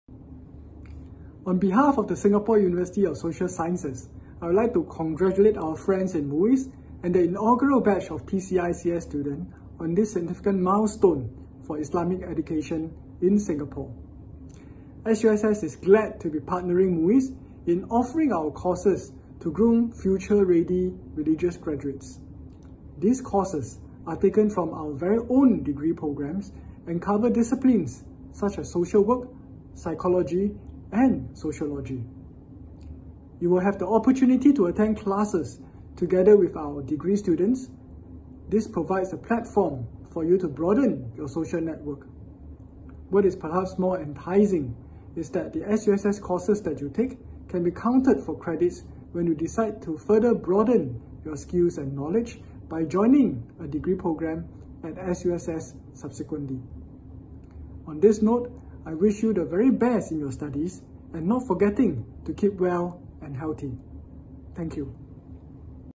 Singapore University of Social Sciences (SUSS)'s Message to the Inaugural Cohort of the PCICS
A/P Calvin Chan wishes the newly matriculated Asatizah of the PCICS all the best in their upcoming journeys!